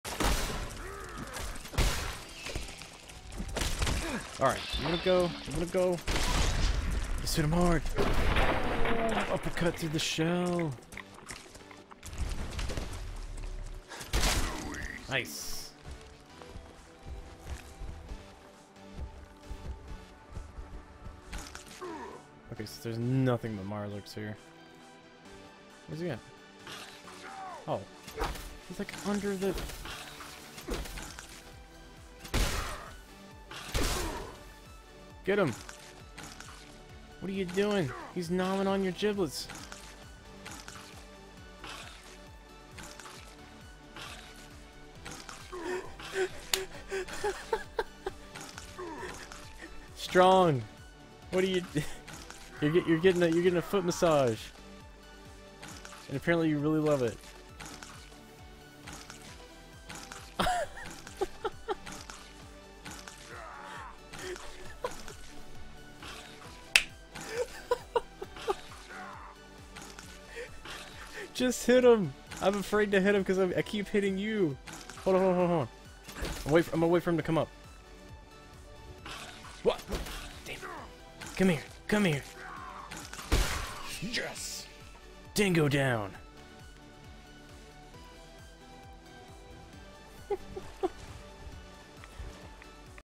Strong Likes Foot Massages
The moment I found out strong was a masochist.